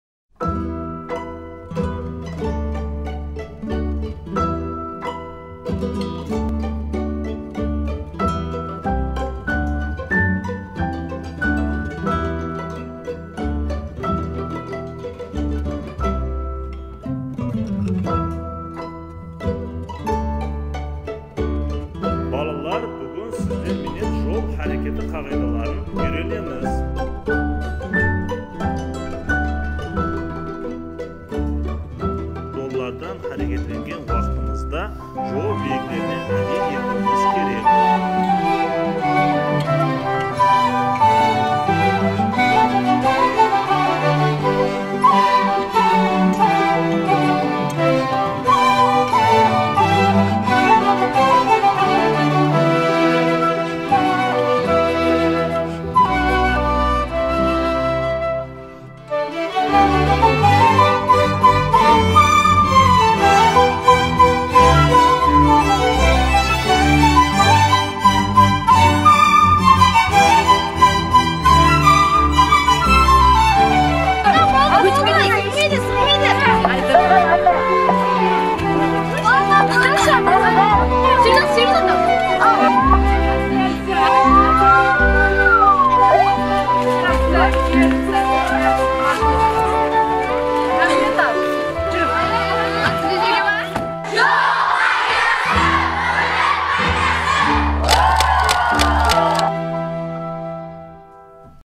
Bolalarga yo‘l harakati qoidalari bo‘yicha tushuntirish ishlari olib borildi
Qoraqalpog‘iston Respublikasi IIV YHXB xodimlari tomonidan Taxiyatosh tumanida joylashgan «Oydin yo‘l» bolalar dam olish maskanida, yo‘l harakati qoidalari bo‘yicha tushuntirish ishlari olib borildi